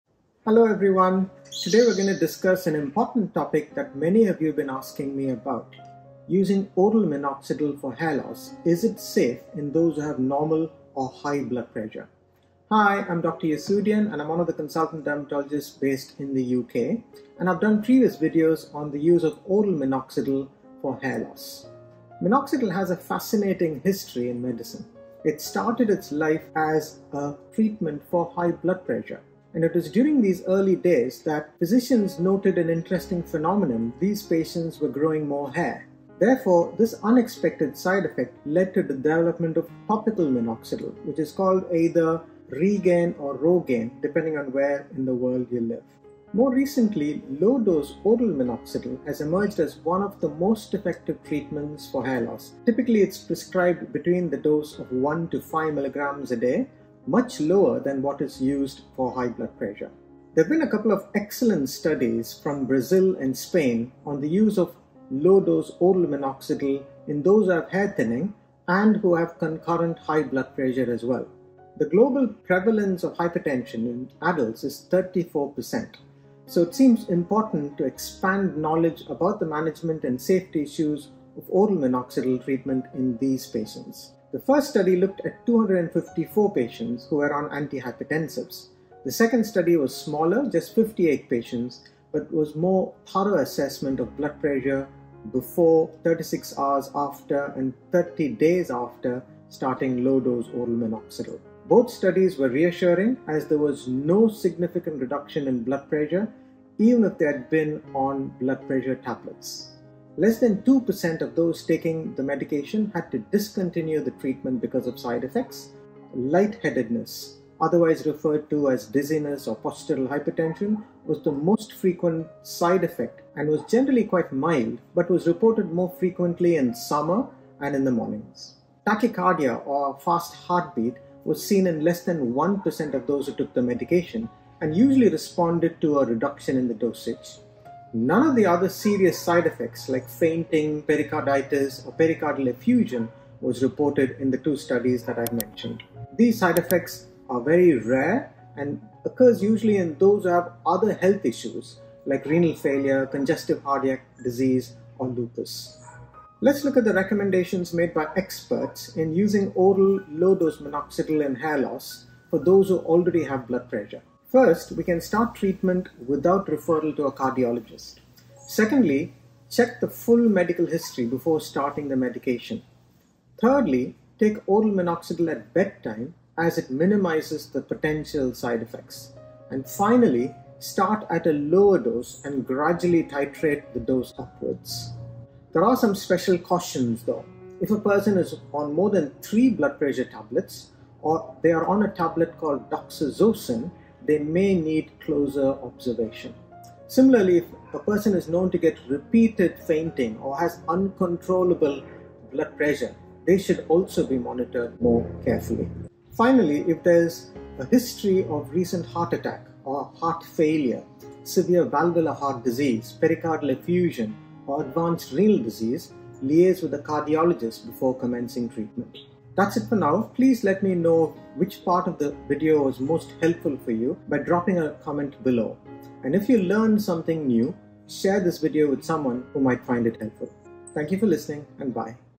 Minoxidil for hair loss | can we use in those with blood pressure?
Oral minoxidil has been used successfully for hair loss due to various causes. It has a good side effect profile. As it was originally a medication for high blood pressure, there are questions on its safety in those who are already on blood pressure medications and heart issues. Two recent studies have looked at this topic in depth and have come to the conclusion that oral minoxidil is safe to use in those who have blood pressure and also in those on medications for blood pressure. A few cautions are advised, which is discussed in the video.

Safety of Low-Dose Oral Minoxidil in Patients With Hypertension and Arrhythmia: A Multicenter Study of 264 Patients. J. Jimenez-Cauhea, R. Pirmezc, P. Müller-Ramosd, D.F. Meloe, D. Ortega-Quijanoa, O.M. Moreno-Arronesa, D. Saceda-Corraloa, et al.

Prospective evaluation of blood pressure, heart rate, and side effects in hypertensive patients using low- dose oral minoxidil for hair loss. Paulo Mu€ller Ramos, Daniel Fernandes Melo,  Bevin Bhoyrul, Baltazar Dias Sanabria, Juan Jimenez- Cauhe, MD, David Saceda-Corralo, MD, Prateek Sharma, Monica Ramos,  et al.